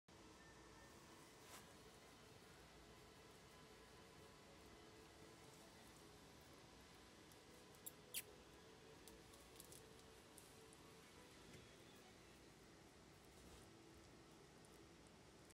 Cute havachons playing together
these two little brother are looking for a new home